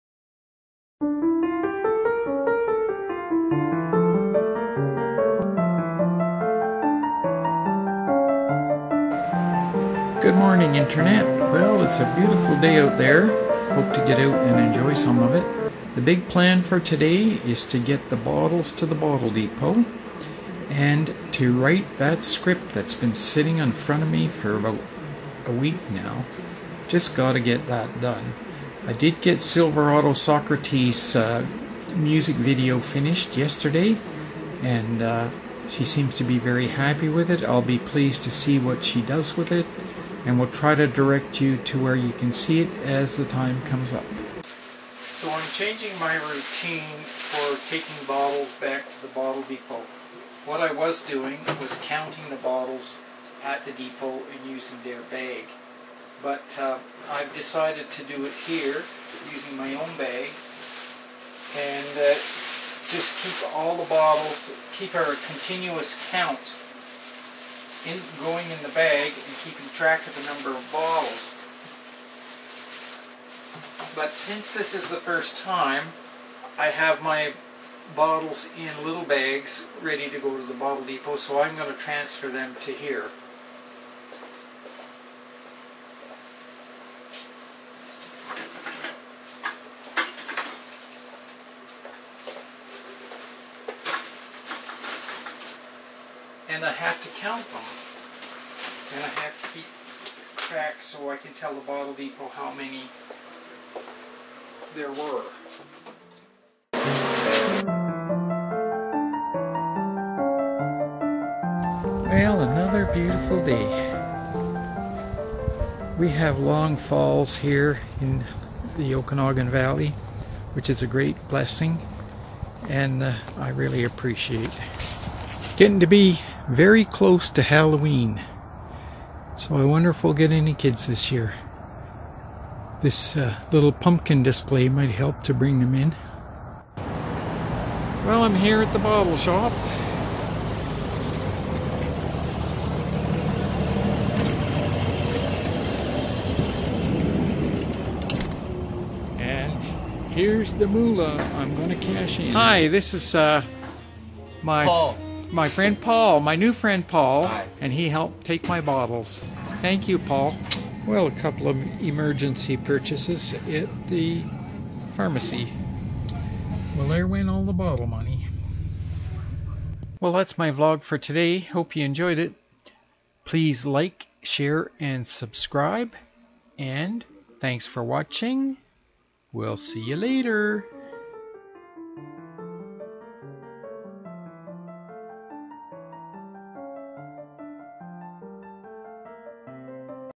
Fred's Vlog
Bottles, Bottles everywhere. I'm trying to be more efficient in my recycling methods.